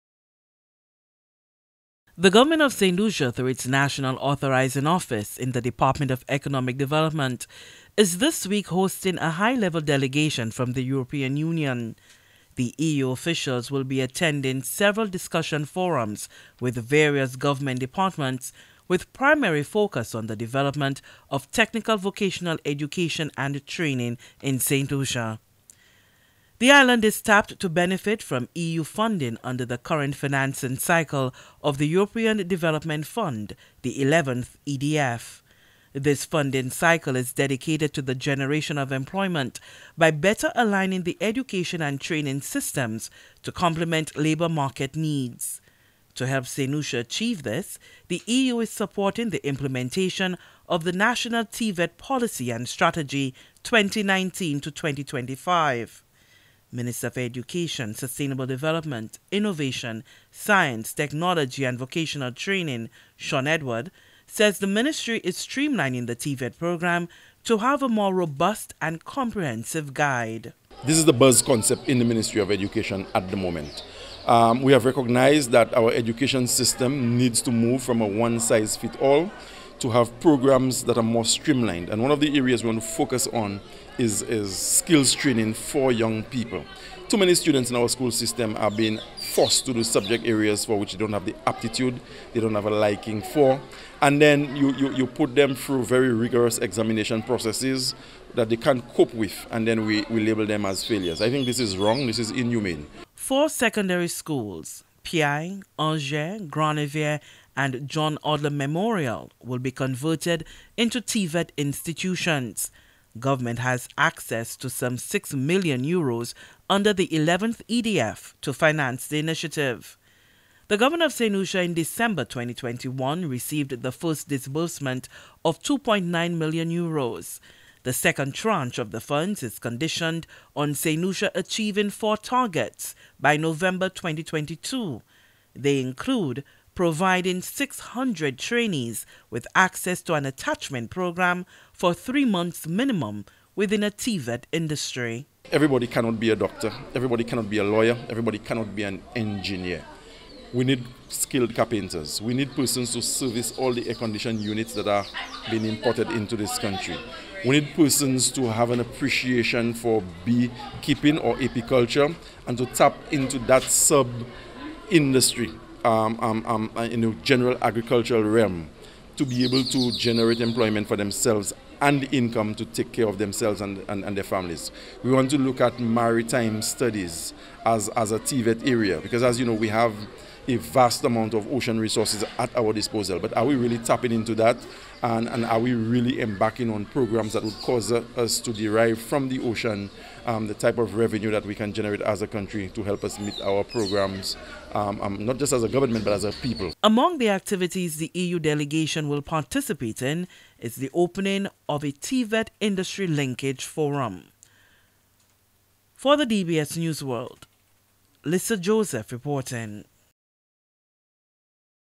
Several Schools to Be Converted to TVET Centres
Education Minister, Shawn Edward says heavy emphasis is being placed on developing a modern curriculum for technical, vocational training in Saint Lucia to meet the demands of the changing job market. Four secondary schools have been identified for conversion into TVET learning institutions. The Education Minister along with technocrats from his Ministry will be discussing the proposal with donor representatives from the European Union.